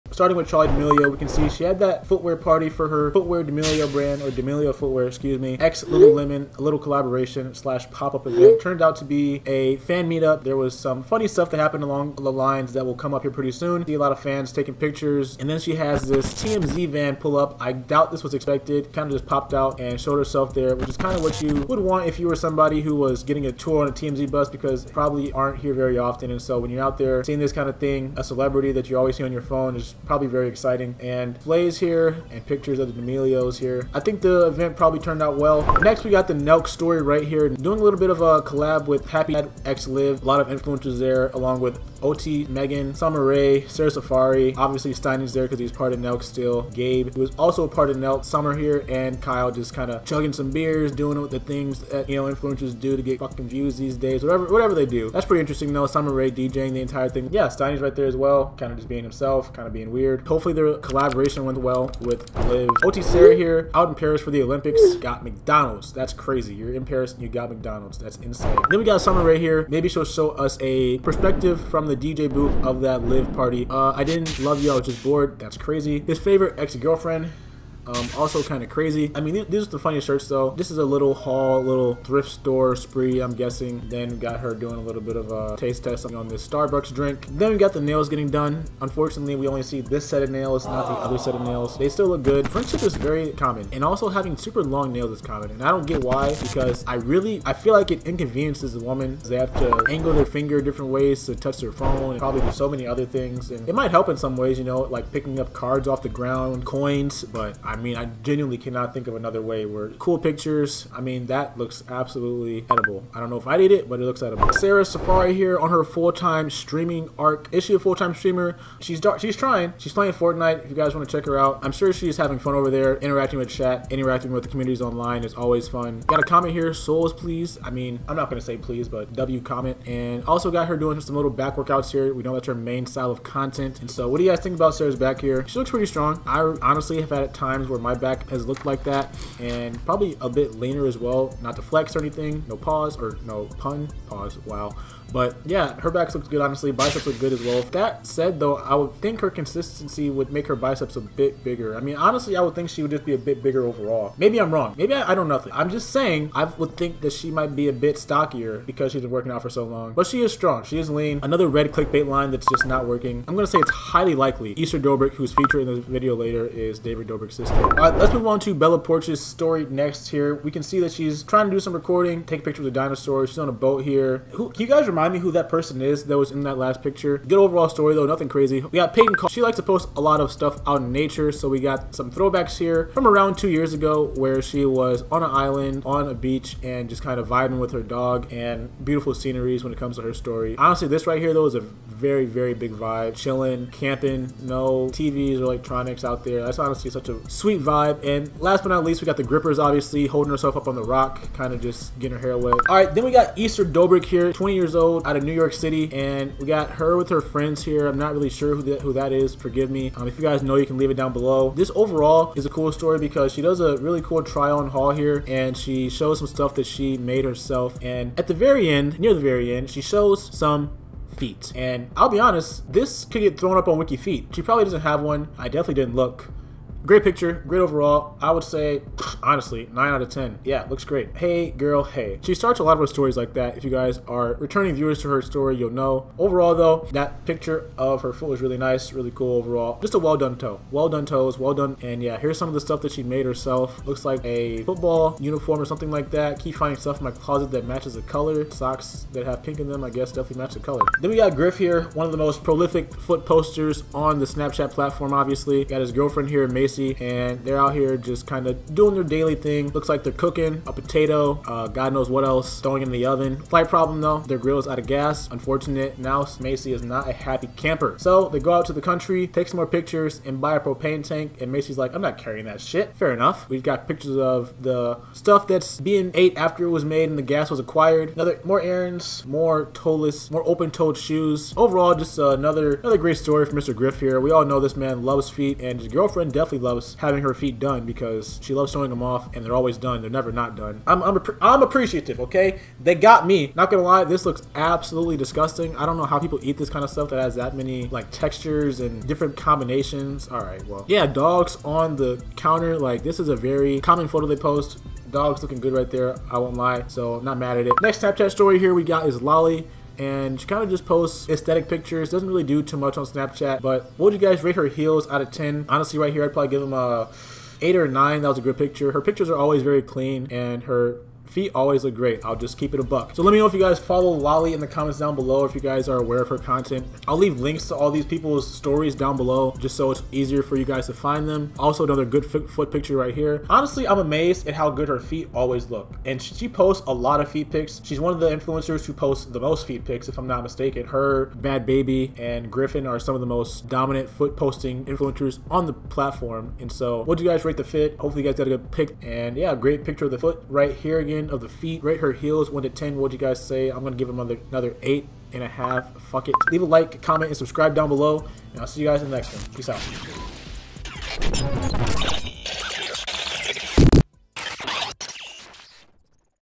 Breaking Down SnapChat Celebrities Showing Their Feet
Join me as I dive into the latest Snapchat stories from Charli D'Amelio, Sara Safari, Ester Dobrik, and more, highlighting all the foot-related content I found. Discover the quirky trend of celebrity feet pics on Snapchat!

If you're reading this. thank you❤❤❤